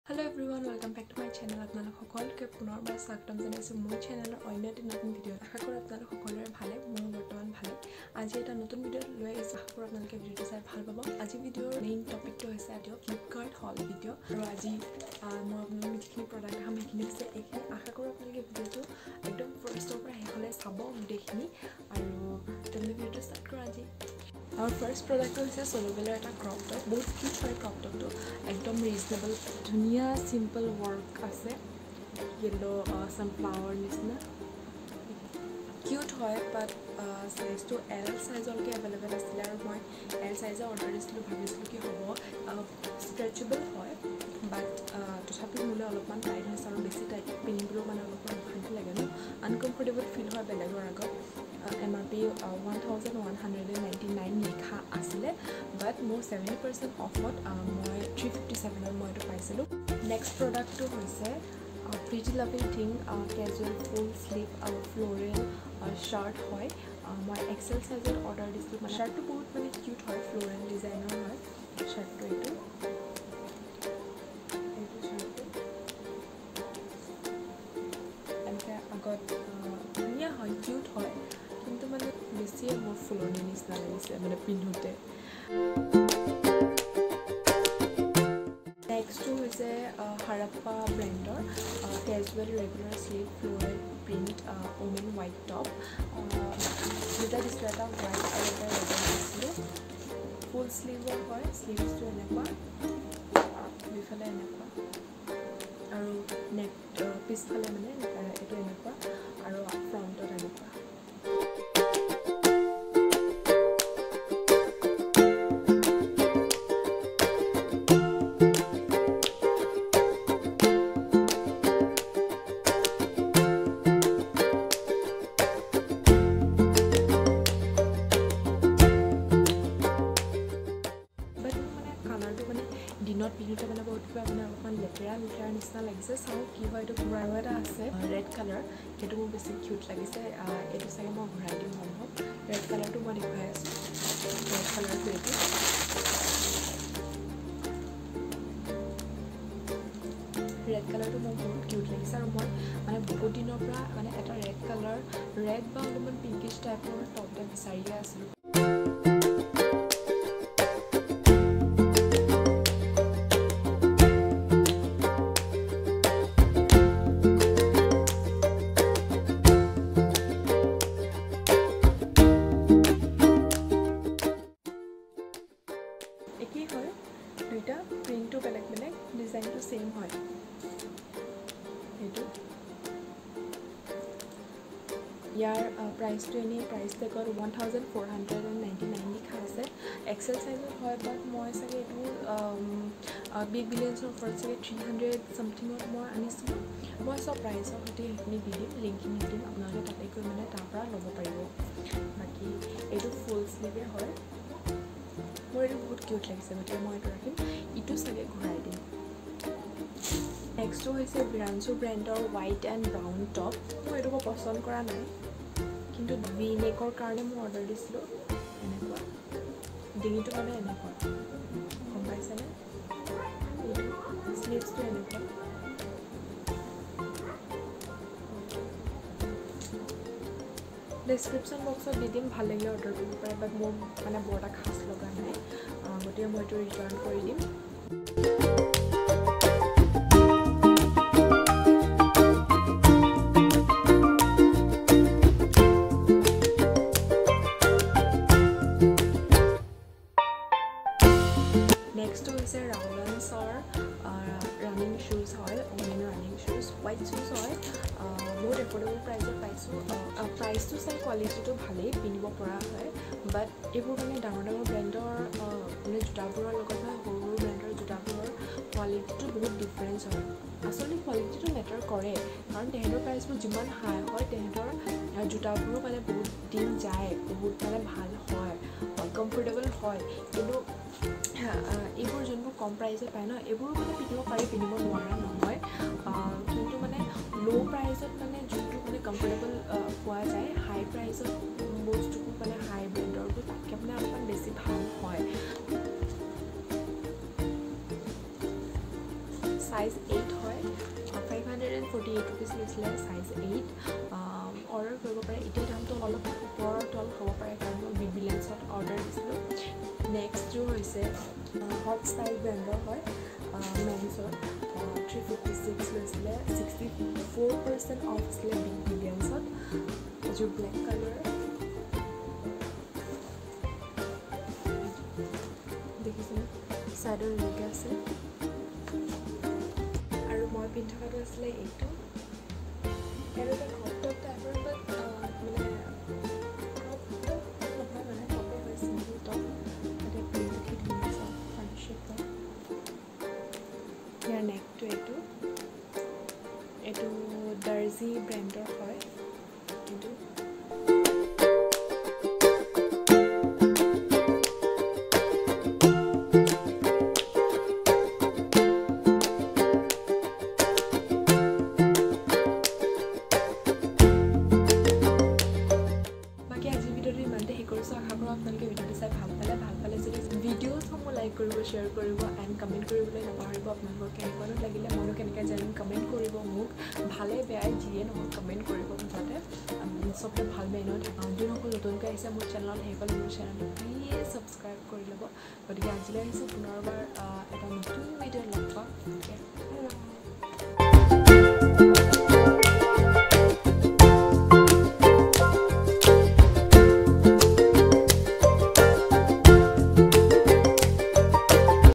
FLIPKART HAUL 🤍| BIG BILLION SALE | PUJA OFFER! 🤩
1) Sollobell white crop top -

2) Harpa floral print woman pink and white top -

Pink - Take a look at this HARPA Casual Floral Print Women Pink Top on Flipkart

White - Take a look at this HARPA Casual Floral Print Women White Top on Flipkart

3) Tandul sleeveless printed pink top - Take a look at this TANDUL Casual Printed Women White, Pink Top on Flipkart

4) Pretty loving thing sleeves floral pink shirt - Take a look at this PRETTY LOVING THING Casual Full Sleeve Floral Print Women White Top on Flipkart

5) White Shoes - Take a look at this Rowlans Running Shoes For Women on Flipkart

6) Black Shoes

7) Veeranshu brown top